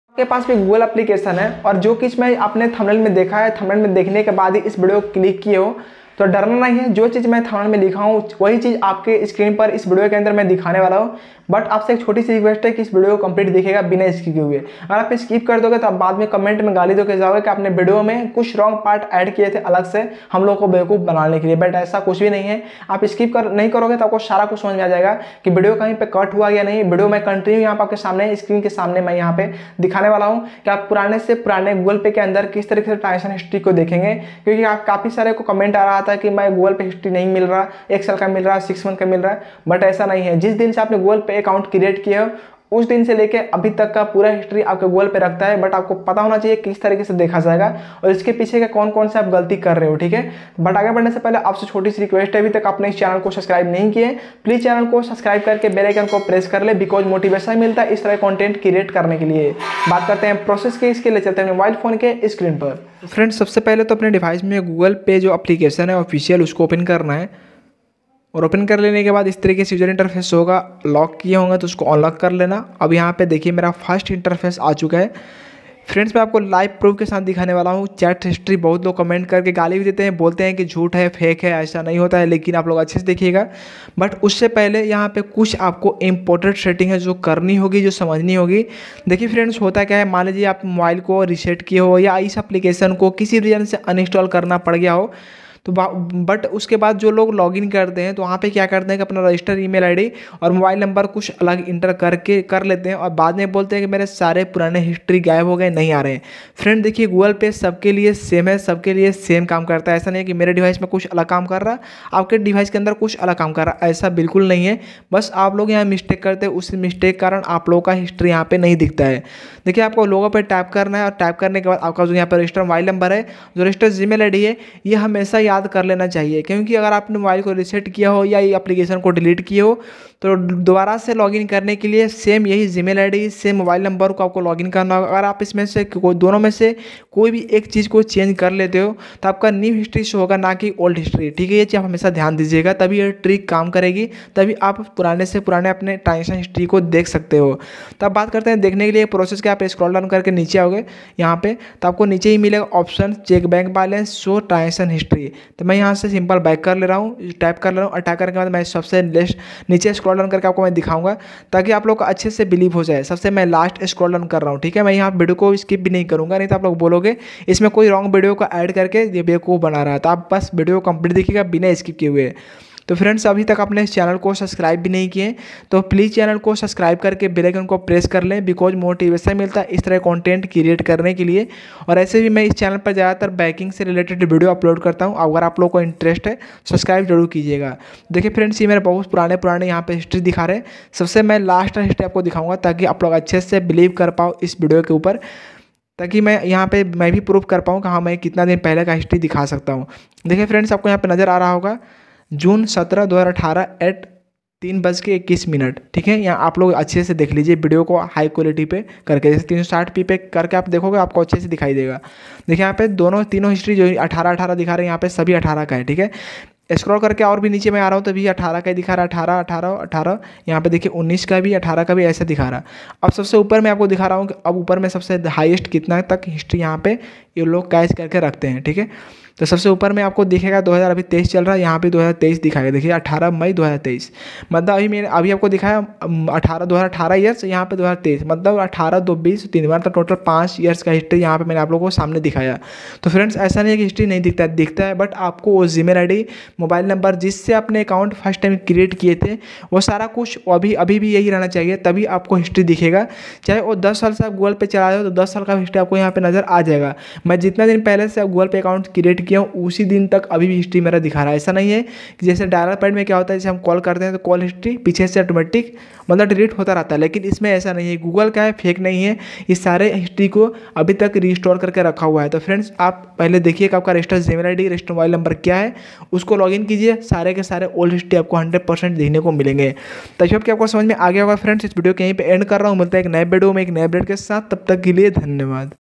Google Pay सालो पुराने Transation history निकालना सीखे।
Hii Friends 🙏
I'm Akil

About This Video 👉 Dosto Aaj Es Video Me Maine Bataya Hai Ki Agar Apke Pass Google Pay Application Hai Or Aap Google Pay Ka Old Transaction History Dekhna Chahte Hai To Aap Es Video Ko Bina Skip Kiye Pura Jarur Dekhiyega